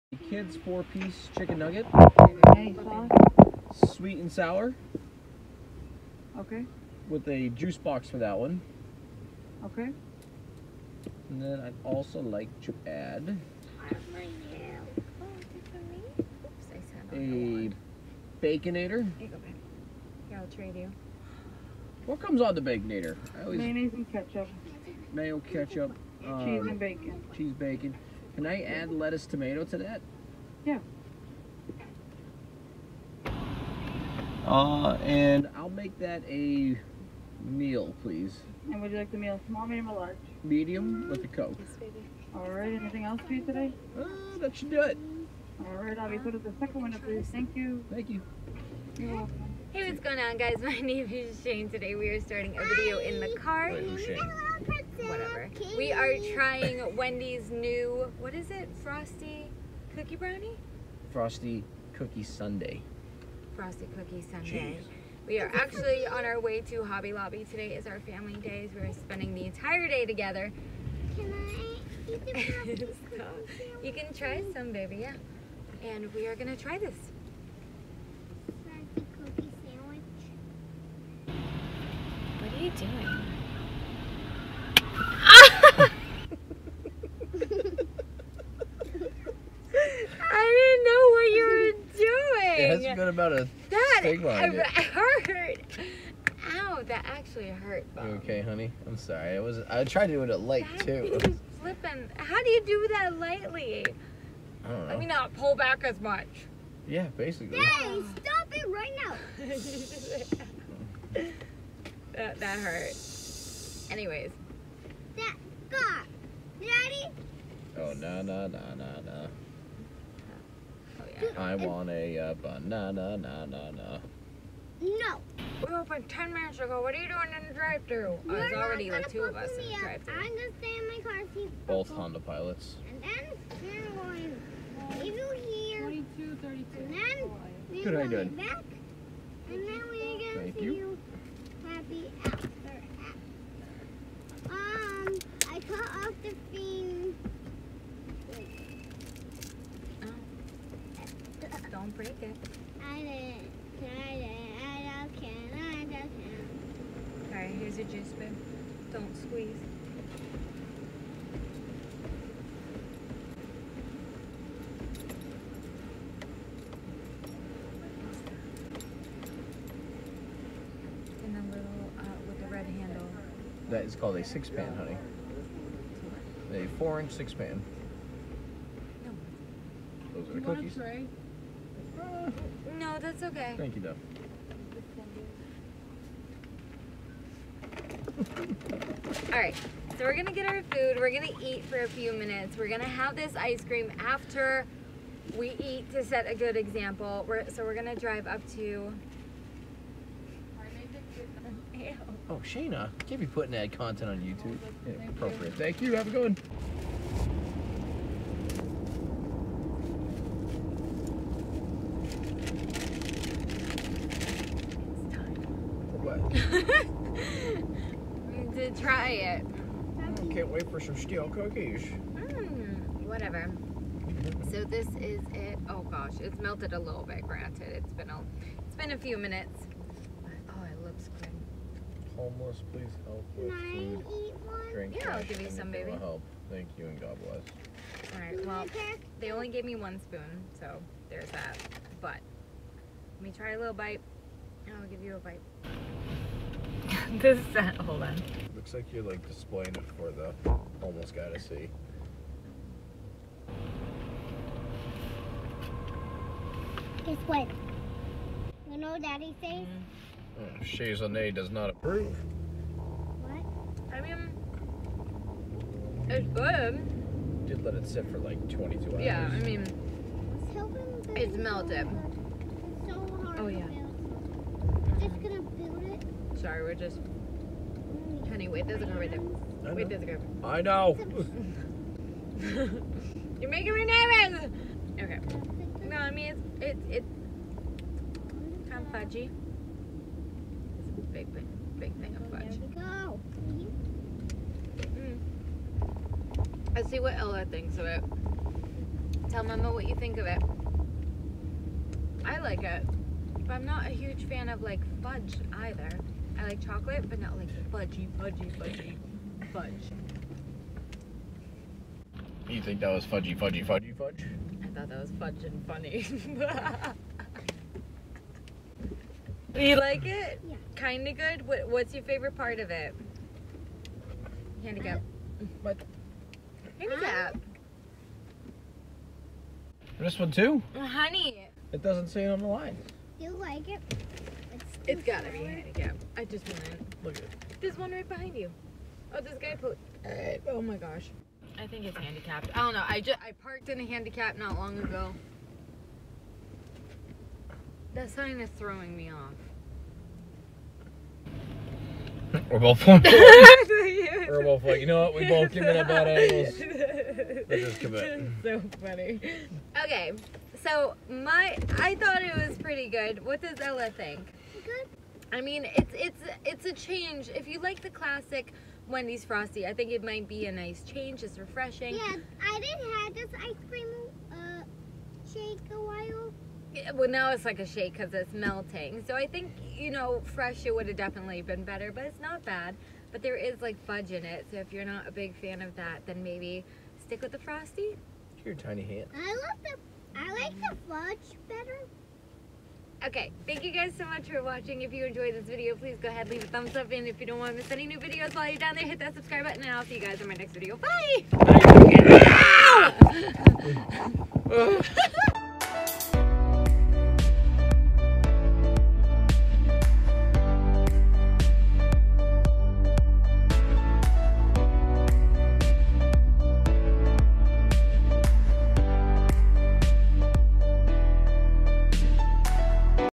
TRYING THE **NEW** WENDY'S FROSTY COOKIE SUNDAE
In today's short video we take a trip through a Wendy's drive thru to put their new Frosty Cookie Sundae to the test! Have you tried it yet? If you have, what did you think?? Hope you enjoyed hanging out with us!!

As always,
xoxo Shane

xoxo Shane